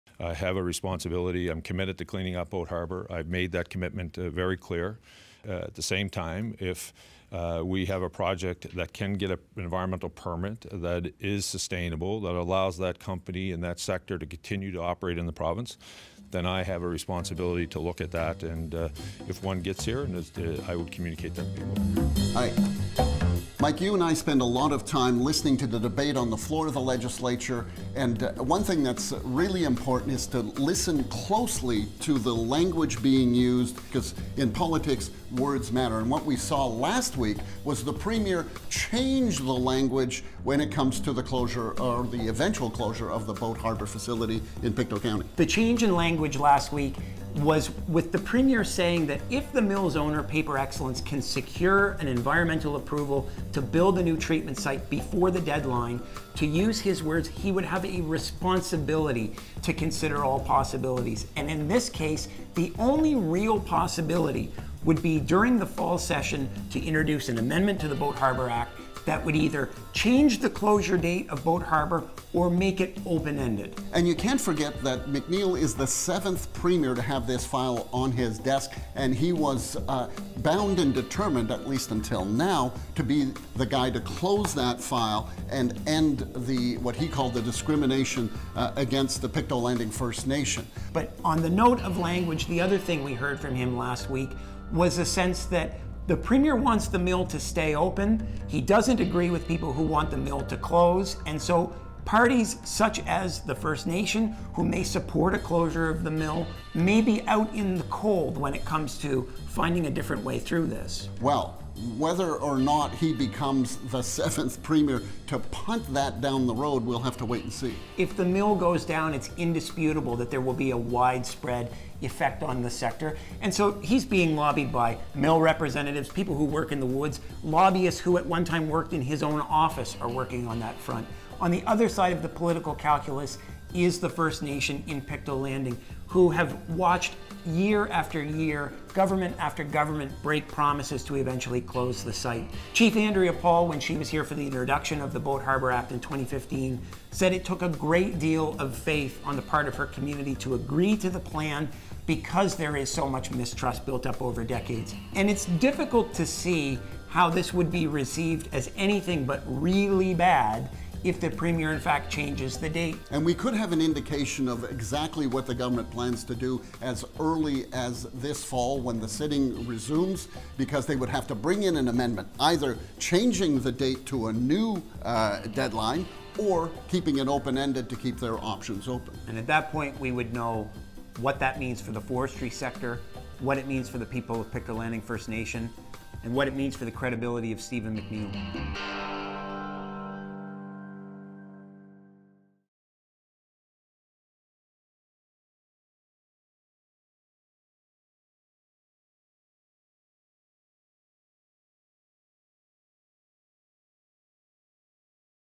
Is the Boat Harbour Act about to be amended?
After not budging on the 2020 Boat Harbour closure date for years, Premier Stephen McNeil's answers during a scrum last week suggest there's a possible scenario in which changes could be made to the Boat Harbour Act this fall.